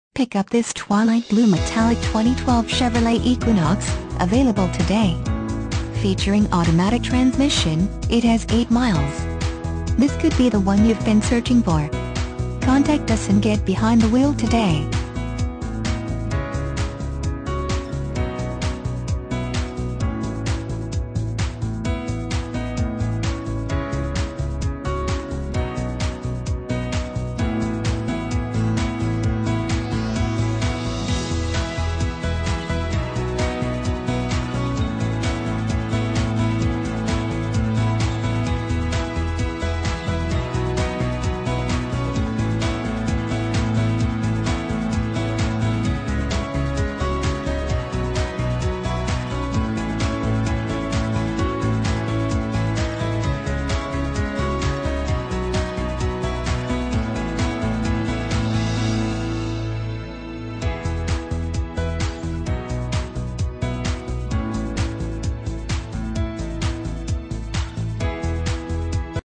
2012 CHEVROLET EQUINOX White Bear Lake, MN 70348
2012 CHEVROLET EQUINOX White Bear Lake, MN
Stock #70348

1801 E County Road F

Pick up this Twilight Blue Metallic 2012 Chevrolet Equinox, available today. Featuring automatic transmission, it has 8 miles. This could be the one you've been searching for.